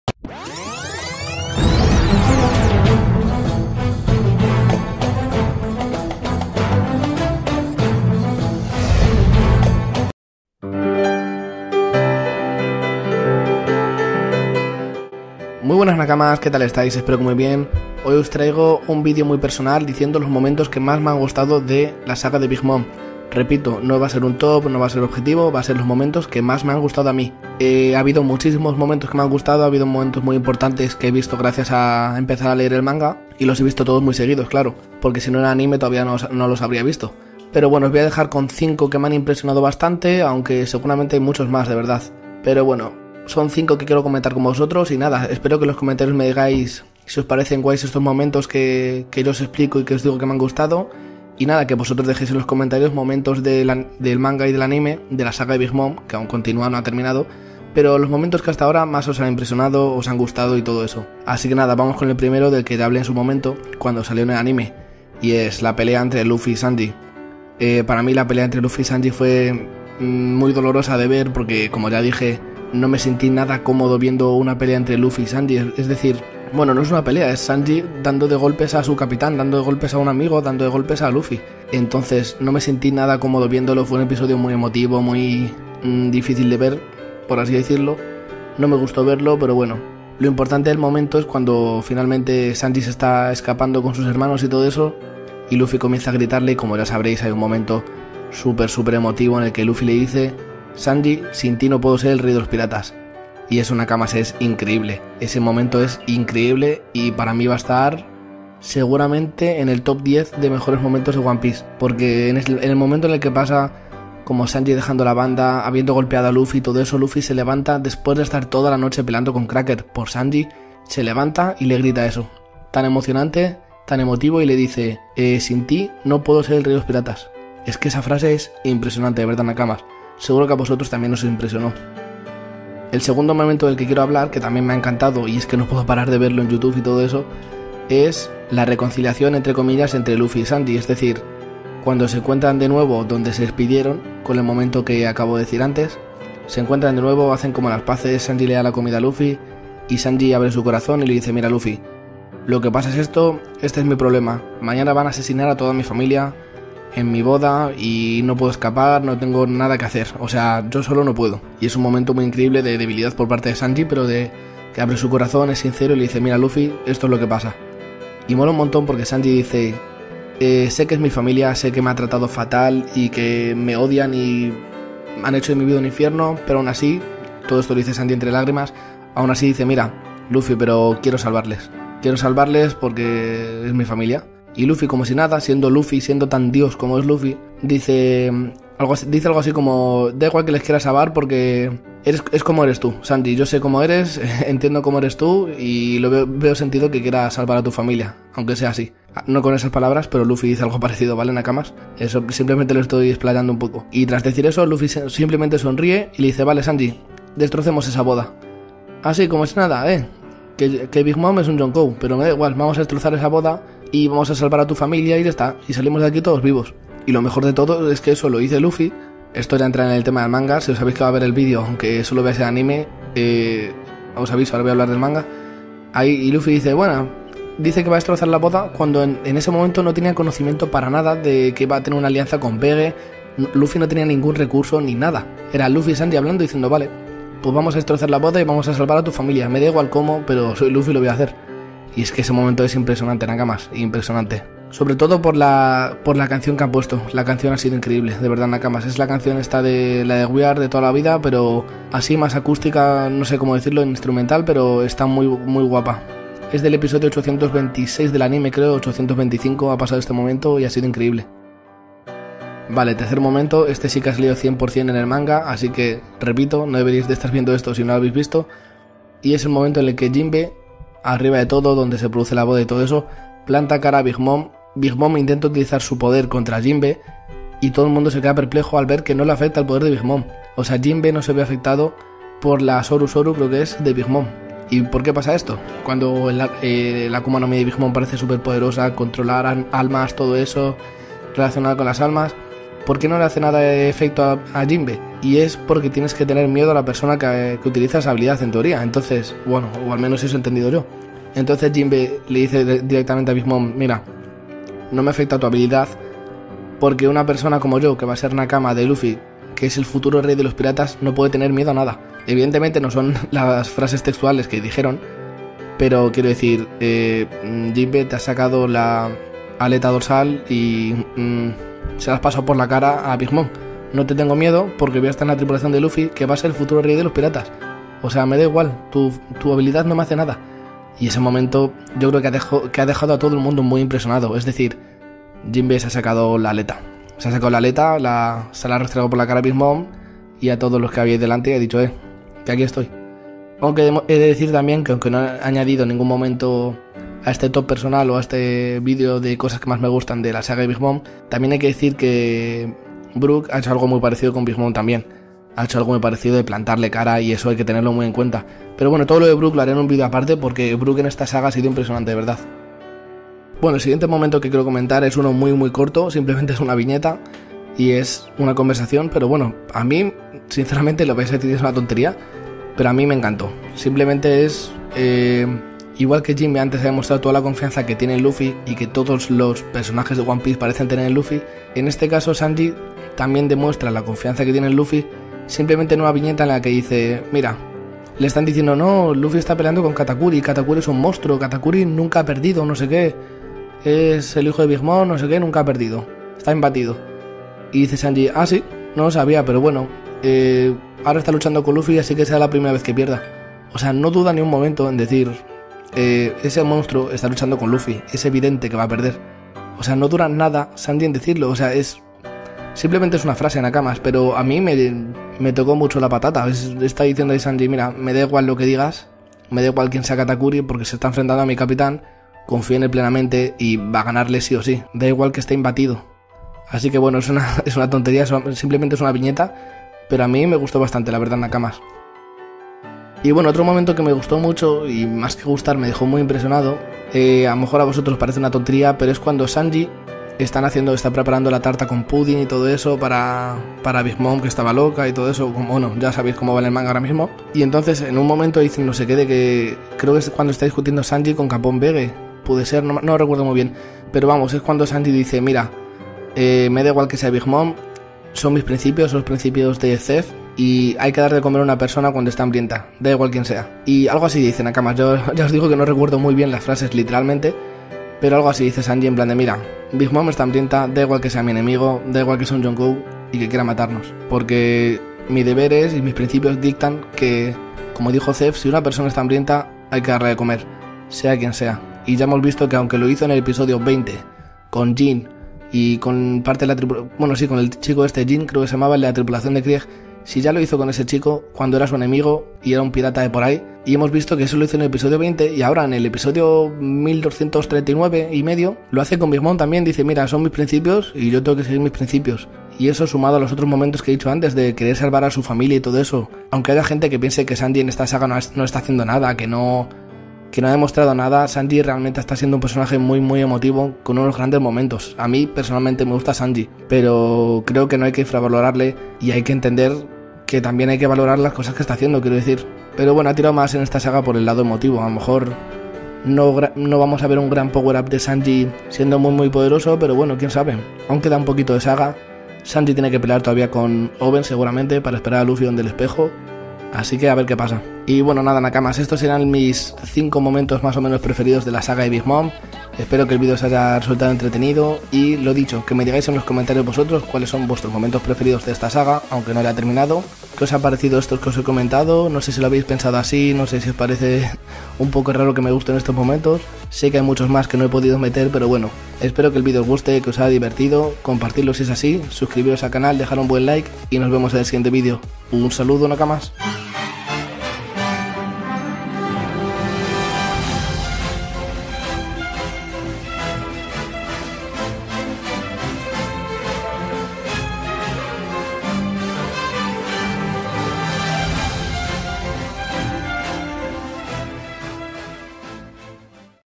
¡MIS 5 MOMENTOS FAVORITOS DE LA SAGA DE BIG MOM! - LUFFY NO MI
¡Nakamas ya sé que la saga aún no ha acabado, pero hay tantos buenos momentos que quería hablar con vosotros sobre los 5 que más me han gustado hasta ahora!